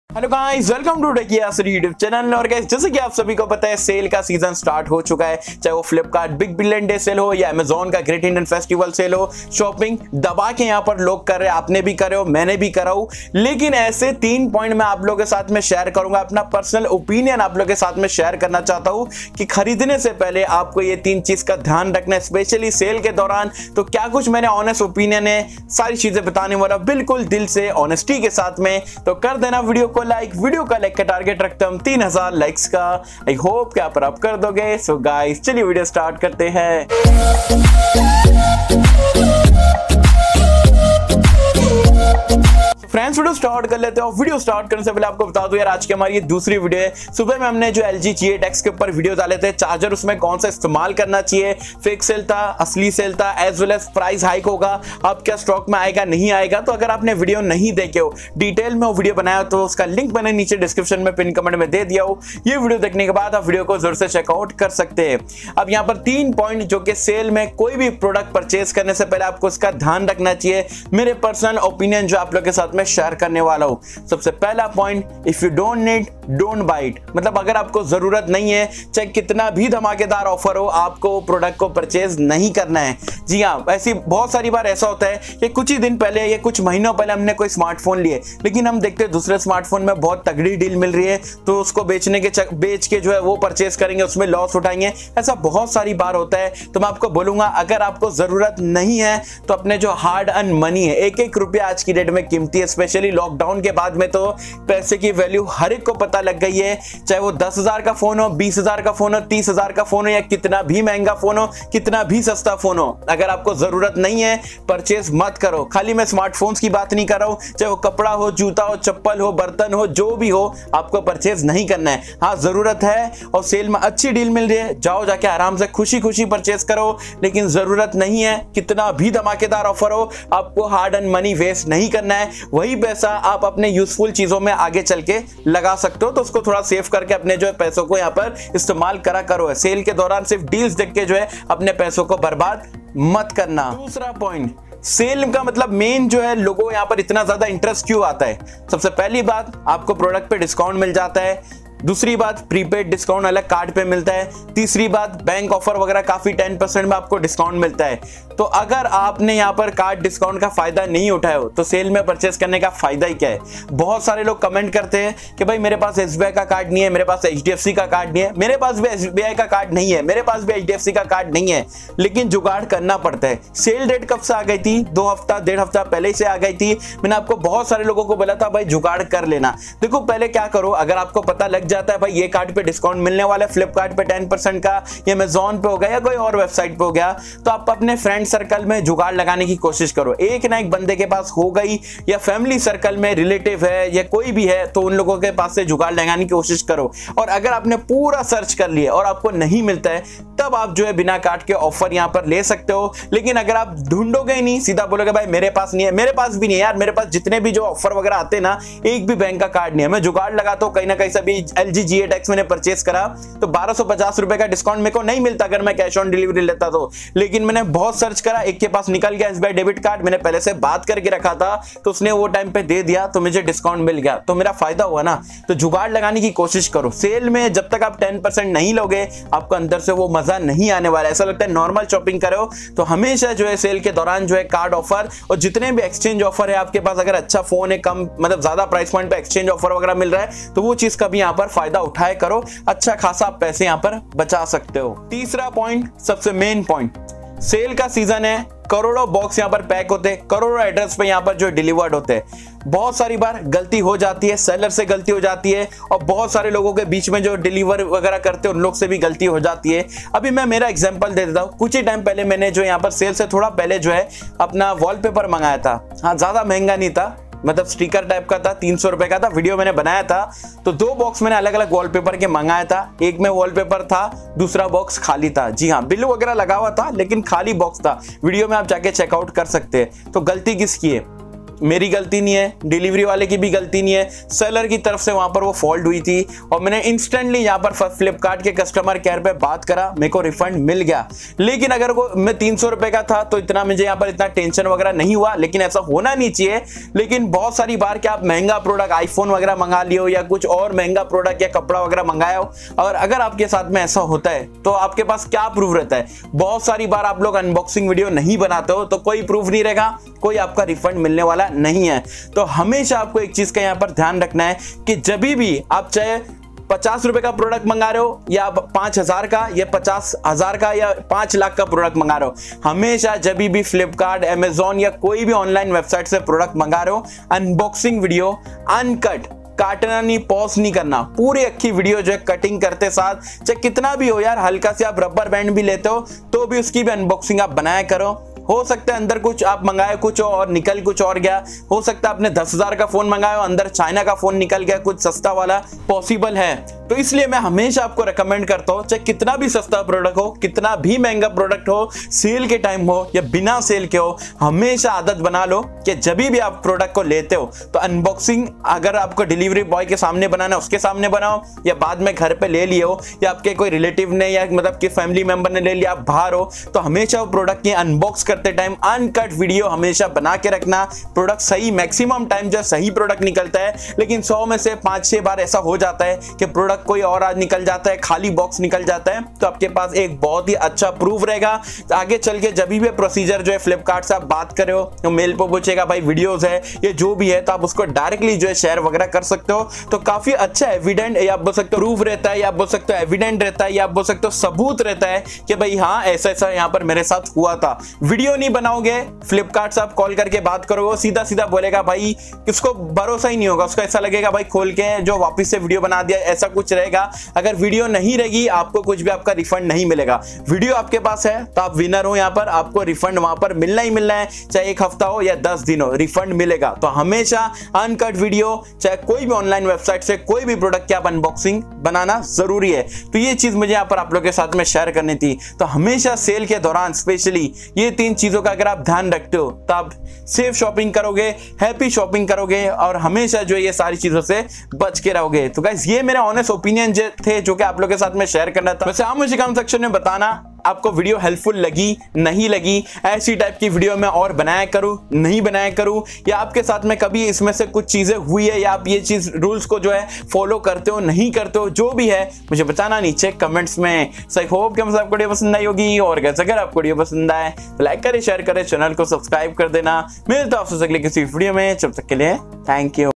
Flipkart Big Billion Days & Amazon Great Indian Festival Sale - 3 Important Points Before Buying 🔥🔥
Lg G8X Fake Sale? LG G8X Price Hike? LG G8X Charger In Box? Flipkart Big Billion Days 🔥🔥

Hello Guys Aj Ki Iss Video Mei Meine Apko 3 Important Points Bataya Hu Jo Apko Sale Mei Koi b Chiz Kharidne Se Pehle Dekhna Chahiye Flipkart big Billion Days & Amazon Great Indian Festival Sale Mei & Apne Honest Opinions Apke Sath Share Kara Hu 👍